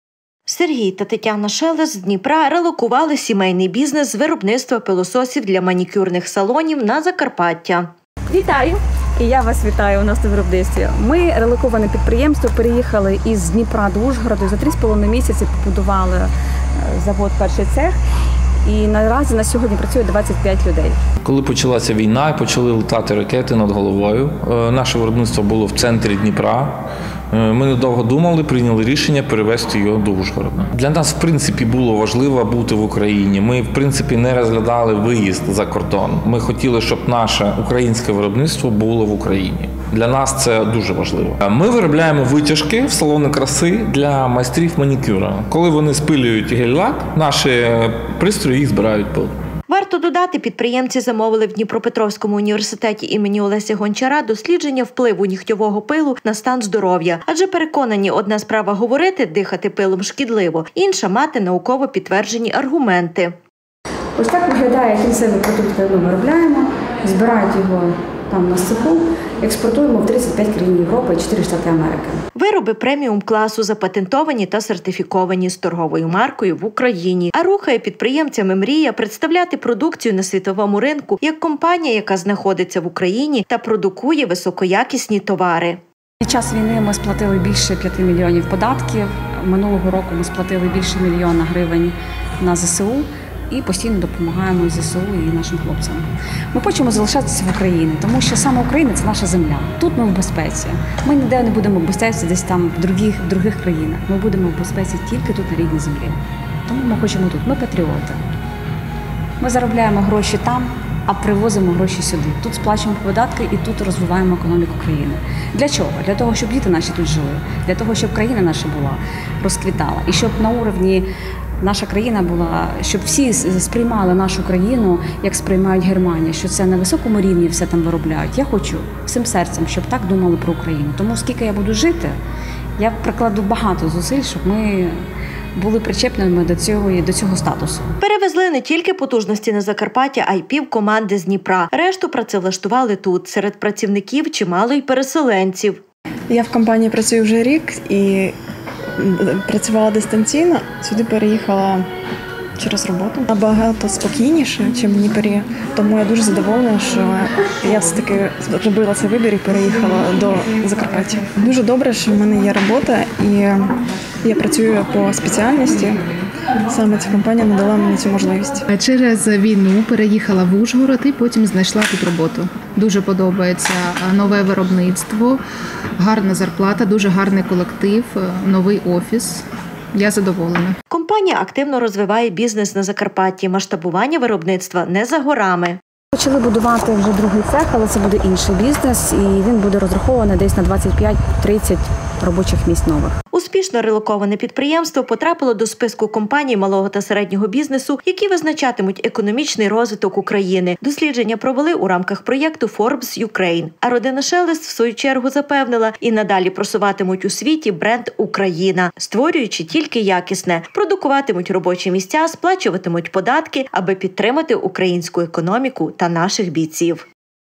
Успішно релоковане
Закарпаття прихистило не тільки сотні тисяч переселенців з різних куточків України, а й цілі підприємства, які успішно розвиваються, створюють робочі місця, сплачують податки, підтримують українську економіку та ЗСУ! Одне з них потрапило до проєкту від Forbes Ukraine  «Next250». Це  список компаній малого та середнього бізнесу, які визначатимуть економічний розвиток України. Мова про сімейну справу родини Сергія та Тетяни Шелест, які перевезли виробнитво пилососів для салонів краси. Сергій та Тетяна Шелест з Дніпра релокували сімейний бізнес з виробництва пилососів для манікюрних салонів на Закарпаття.  Варто додати, підприємці замовили в Дніпропетровському університеті ім. Олеся Гончара дослідження впливу нігтьового пилу на стан здоров’я. Адже перконані,  «Одна справа говорити – дихати пилом шкідливо, інша – мати науково підтверджені аргументи».  Вироби  –преміум класу, запатентовані та сертифіковані, з торговою маркою в Україні. А рухає підприємцями мрія представляти продукцію на світовому ринку, як компанія, яка знаходиться в Україні та продукує високоякісні товари!  Перевезли не тільки потужності на Закарпаття, а пів команди з Дніпра. Решту працевлаштували тут. Серед працівників – чимало й песеленців. Компанія активно розвиває бізнес на Закараптті. Масштабування виробництва - не за горами! Успішно релоковане підприємство потрапило до списку компаній малого та середнього бізнесу, які визначатимуть економічний розвиток України. Дослідження провели у рамках проєкту Forbes Ukraine.  А родина Шелест запевнила, і надалі просуватимуть у світі бренд «Україна», створюючи тільки якісне, продукуватимуть робочі місця, сплачуватимуть податки, аби підтримати українську економіку та наших бійців!